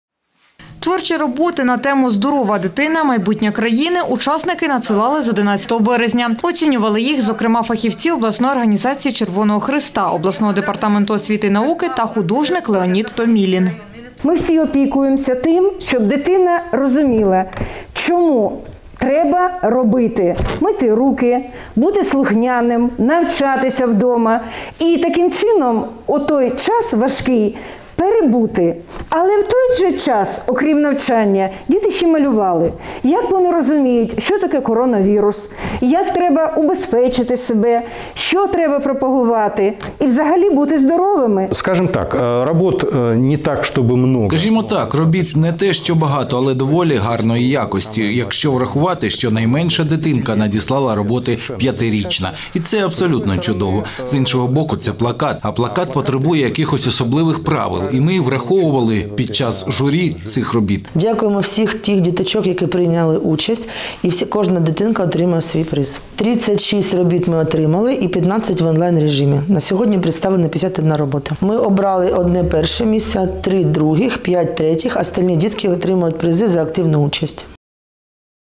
Новини - Конкурс малюнків - 02.06.2020
Творчі роботи на тему "Здорова дитина - майбутнє країни" учасники надсилали з 11 березня. Оцінювали їх, зокрема, фахівці обласної організації Червоного Хреста, обласного Департаменту освіти і науки та художник Леонід Томілін.